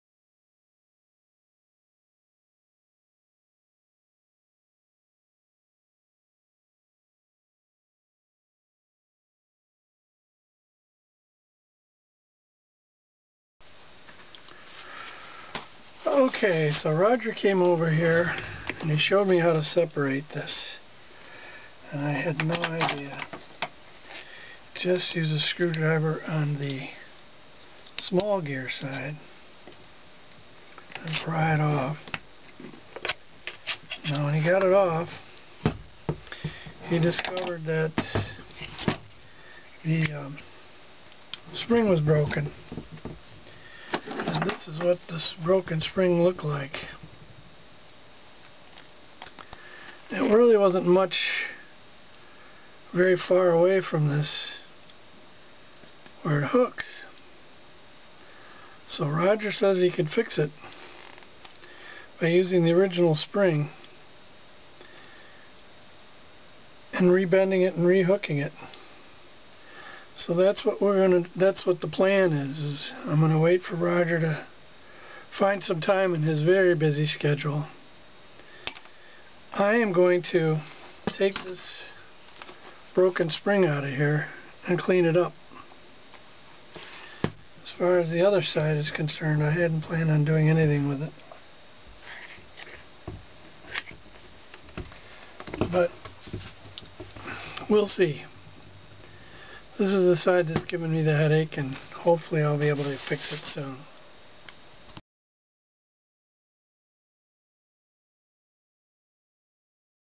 Edison Spring Repair Part II: Revealing the Broken Spring
This short video shows the early steps in the repair process.